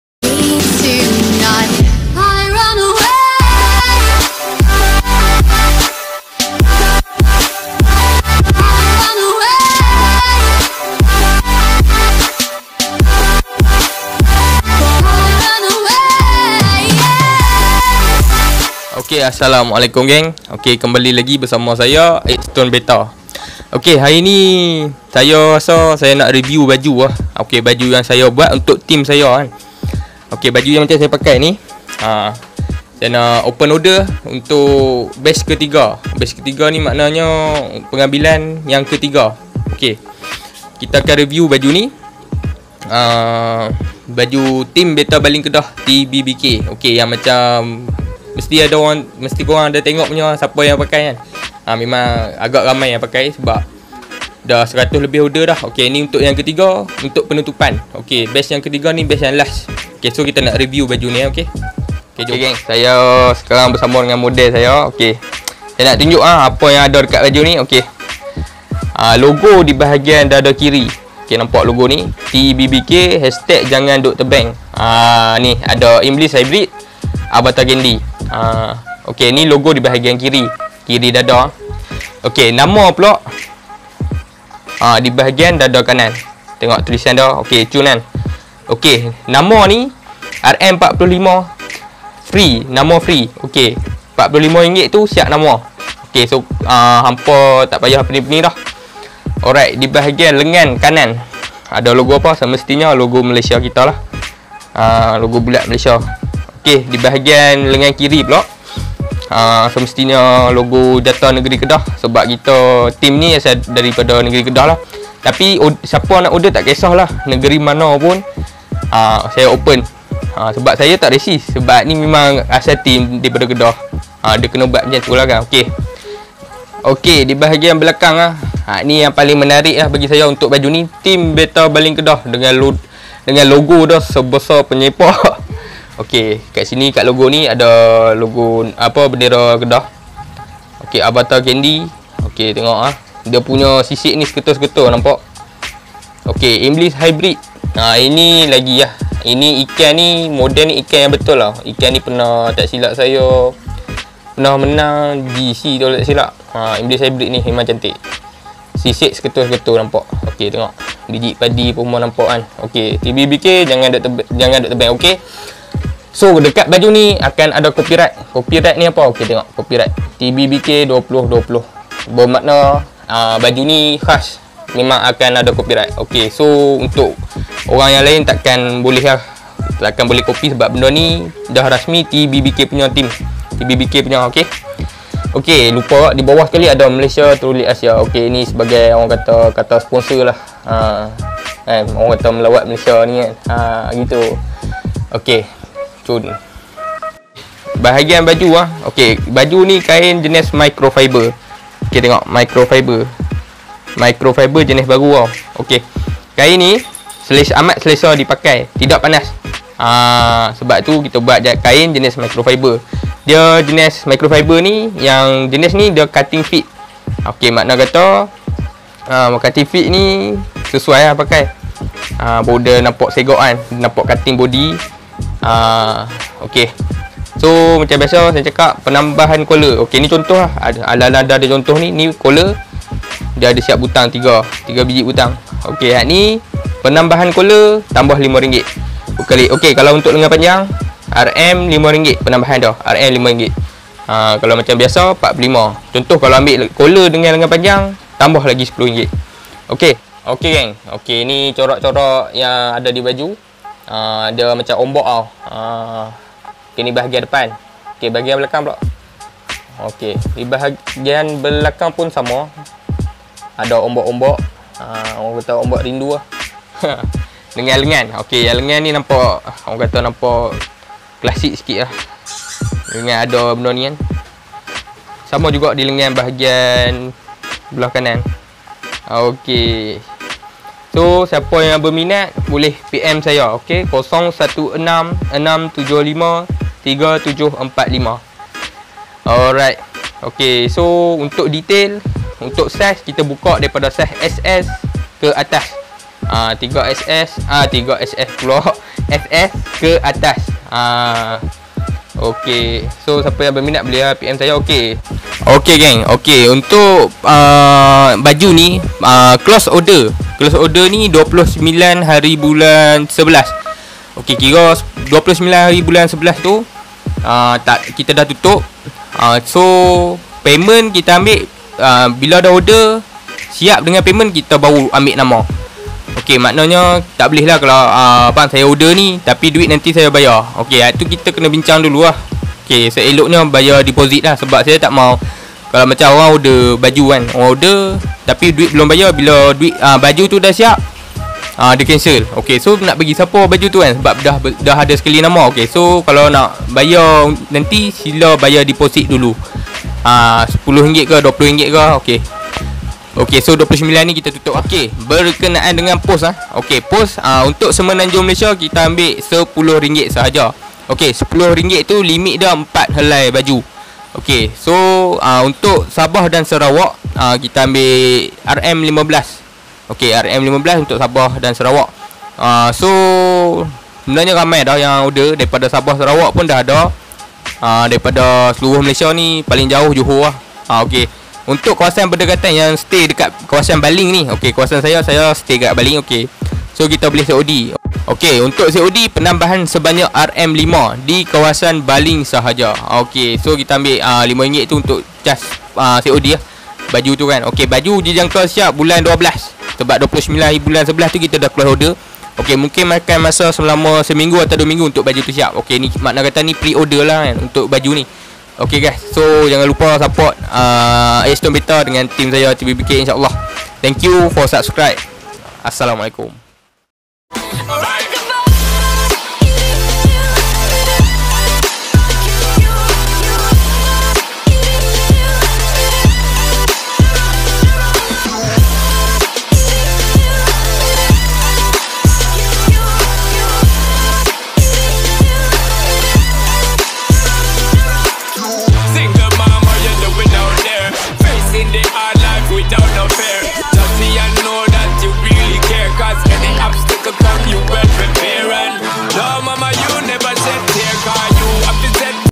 Pre- Order Baju Team Betta Baling Kedah (REVIEW) GRAB IT NOW!
Assalamualaikum bettarians, disini saya ada open pre-order baju Team Betta Baling Kedah (TBBK)

Sehelai:RM45 (Free Name Set) (Size S - XXL)

Add On:
Kolar + RM5
Lengan Panjang +RM5
Size 3XL ke atas +RM10

COD Kawasan Baling, Kedah +RM5
Pos Semenanjung +RM10 (Limit 4 helai)
pos Sabah & Sarawak +RM15 (Limit 3 helai)